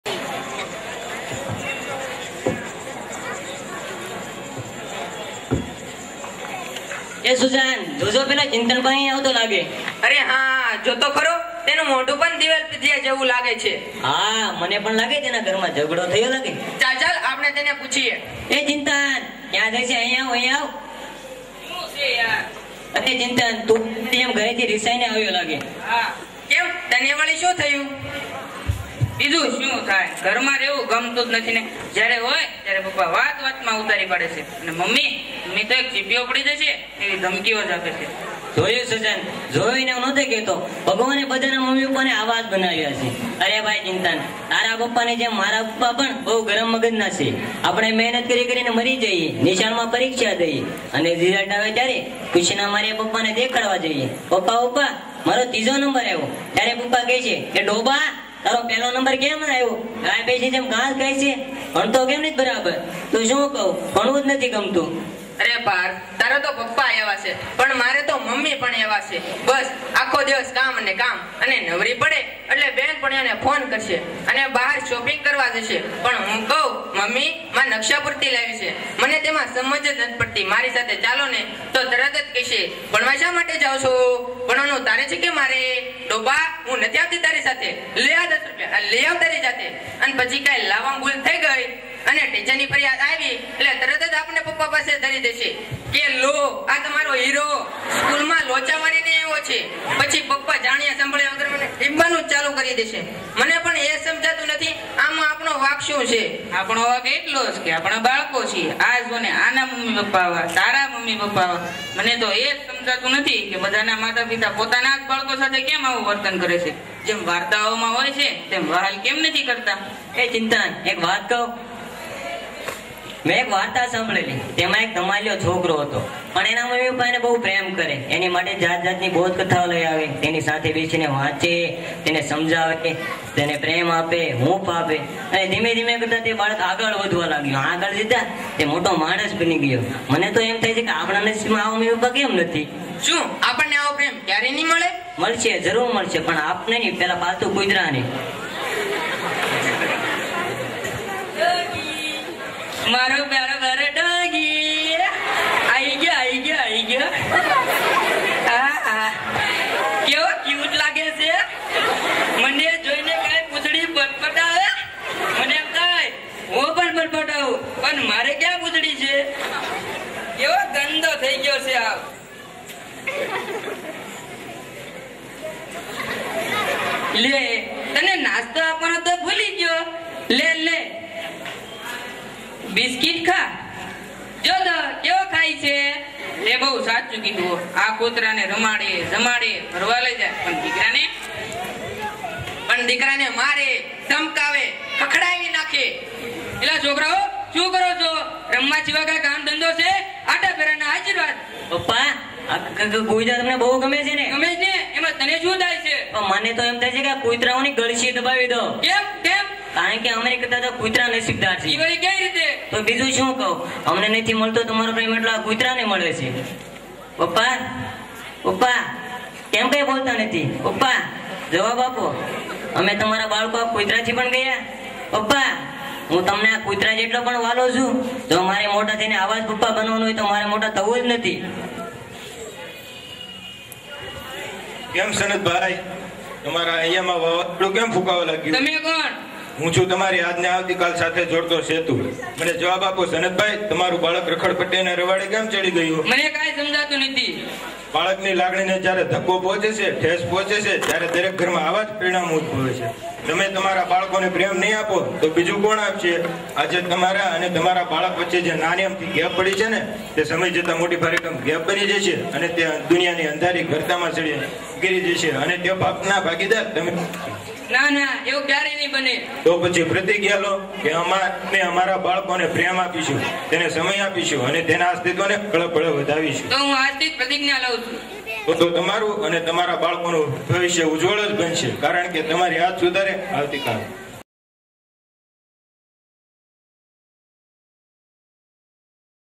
Social Drama | સામાજિક નાટક
Performance by boys on independence day 2015.
Shri K.M. Borda Saraswati Vidyalaya.Surnagar.